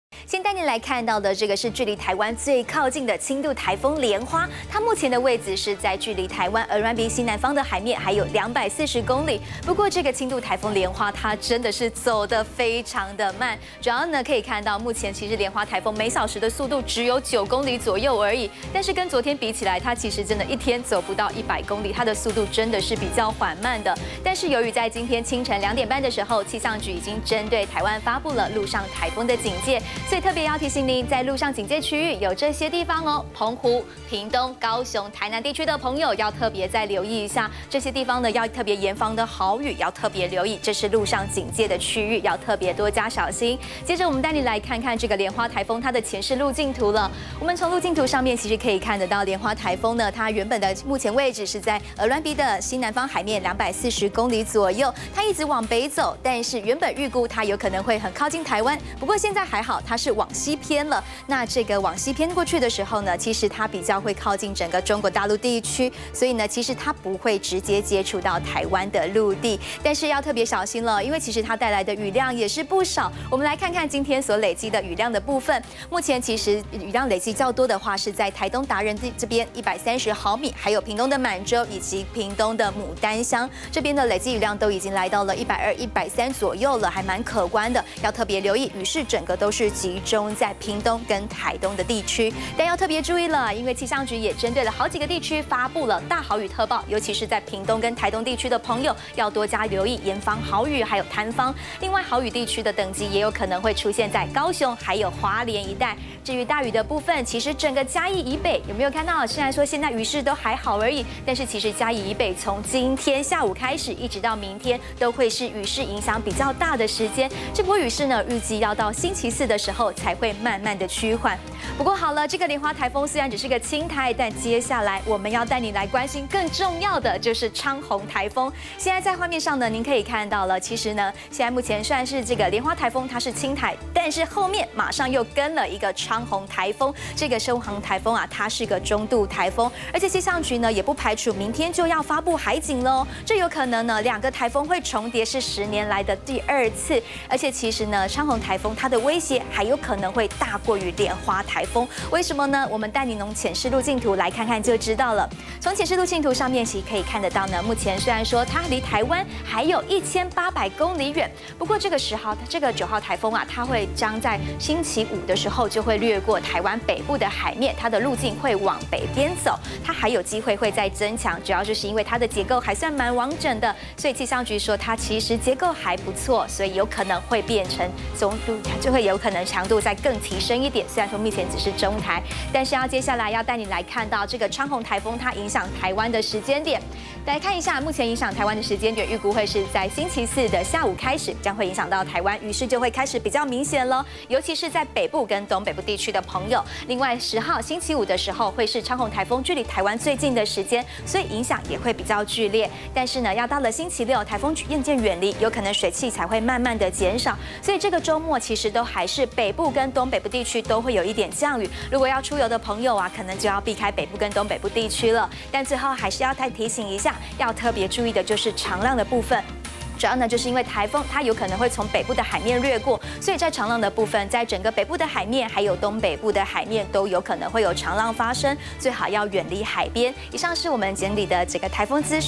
20150707中天新聞 【氣象】蓮花、昌鴻警報「時間恐重疊」 10年來2次
蓮花、昌鴻警報「時間恐重疊」10年來2次，今晚到明天「蓮花颱風」影響最劇烈！「昌鴻颱風」發展穩定，不排除增強可能性，太平洋高壓若南壓「昌鴻」恐成可怕西北颱？